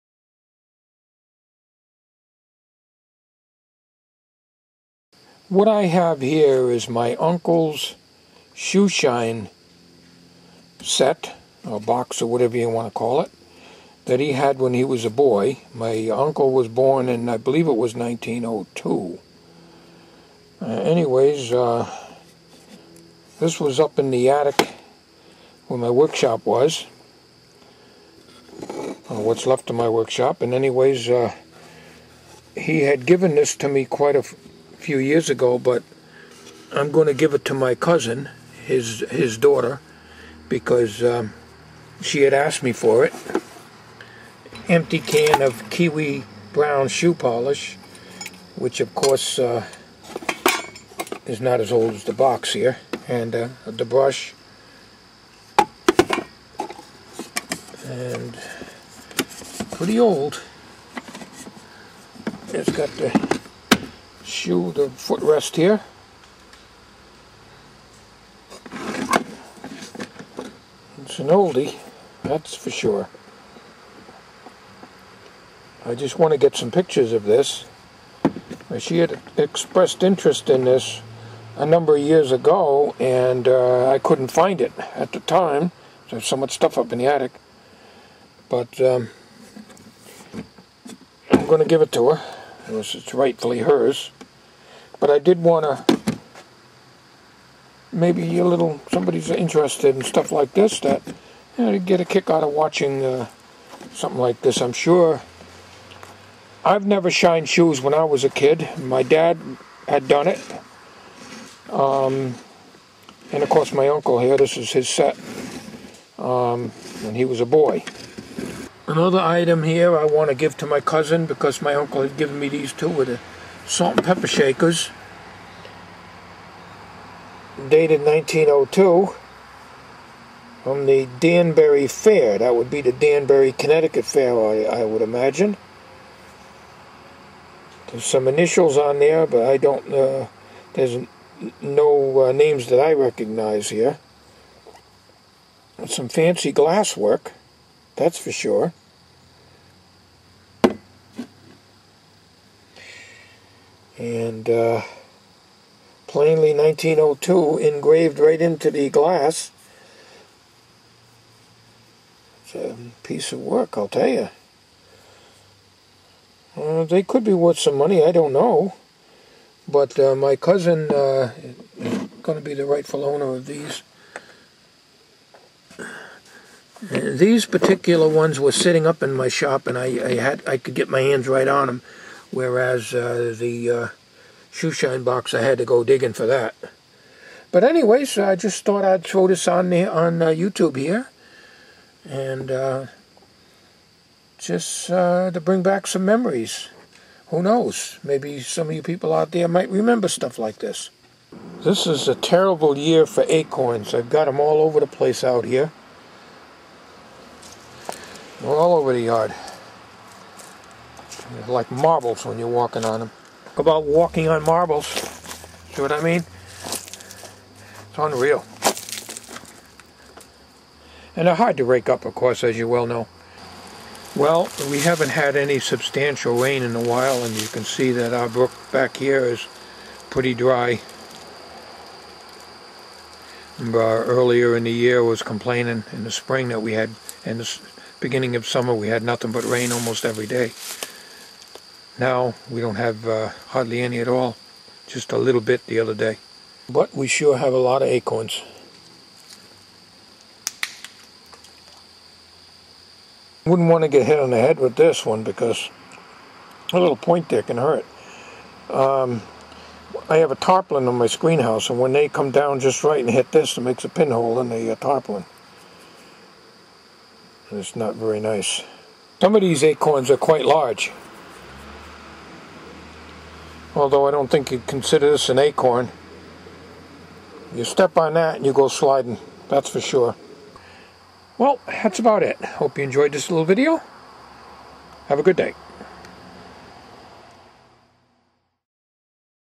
From Antiques To Nuts
This is a short video of two antique items that are in the family.
The first is a shoe shine  box my uncle had used as a kid to make some spending money.
Next is a salt and pepper set dated 1902, just happens that my uncle was born that year.
 I had them in storage and gave them to my first cousin as they were more hers than mine.
As for the nuts. well I had to add some humor to this video.